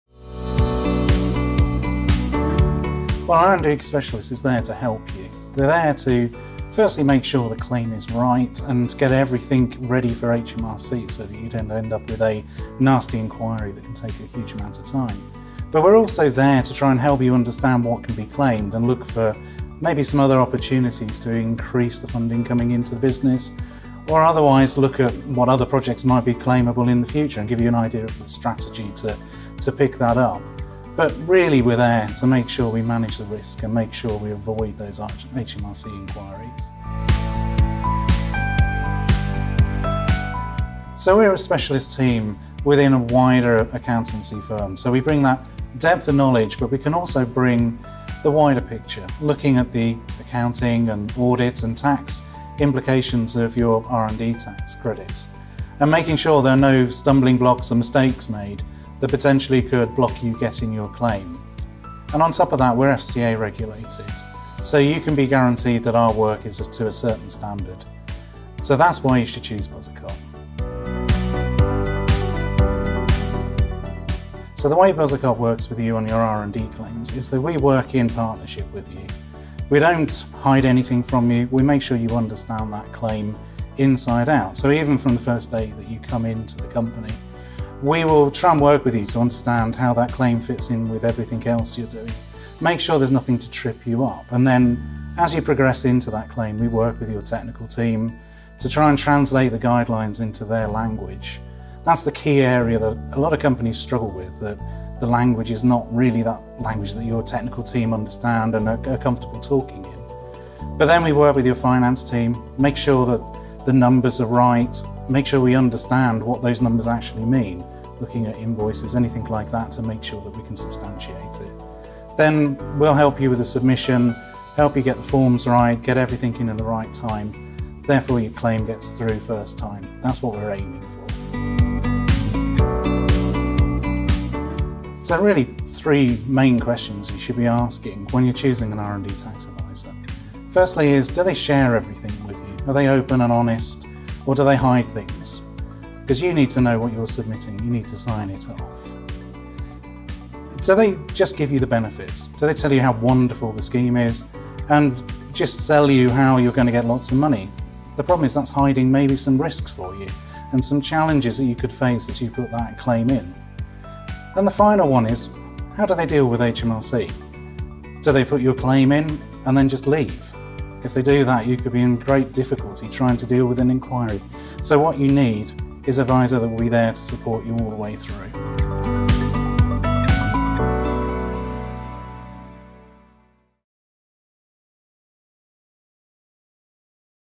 Why choose Buzzacott for R&D support?
Find out everything you need to know about HMRC R&D tax credits with industry expert and R&D Director at Buzzacott, Iain Butler. Join us as Iain breaks down key aspects to help your business thrive.

Topics Covered:

What Can an R&D Specialist Do for Me? (0:02)
Understand the invaluable role an R&D specialist plays in elevating your business through comprehensive insights and strategic guidance.

Why Choose Buzzacott for R&D Support? (0:38)
Iain Butler unveils the unique qualities that set Buzzacott apart, making it your optimal partner for seamless and effective R&D support.

How We Work With You (1:12)
Explore the collaborative approach Buzzacott adopts in working closely with you, ensuring a personalised and tailored experience that addresses your specific R&D needs.

Questions to Ask When Choosing Your R&D Tax Credits Adviser (2:14)
Iain shares key questions to consider when selecting an R&D tax credits adviser, empowering you to make an informed decision for the success of your business.

If you have any questions, or if you’re looking for more information or assistance, please get in touch: